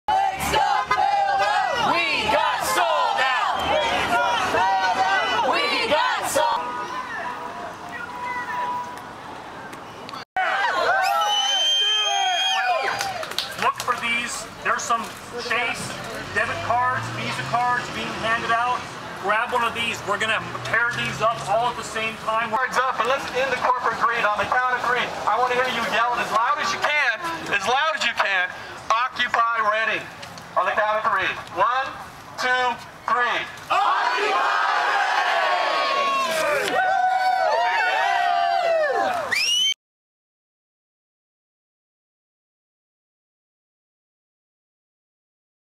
Oct. 15 Occupy Redding demonstration
Some 200 people showed up Saturday for the third - and so far largest - local rally in support of the Occupy Wall Street movement.